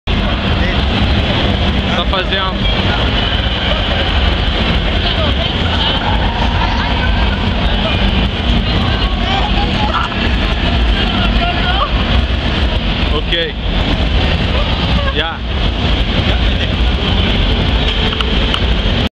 O Macaco da Plaza Forum - Viagem Mundial
Esse macaco é meio doido, depois que ele sobe em você, ele começa a procurar por possíveis coisas comestíveis na sua cabeça.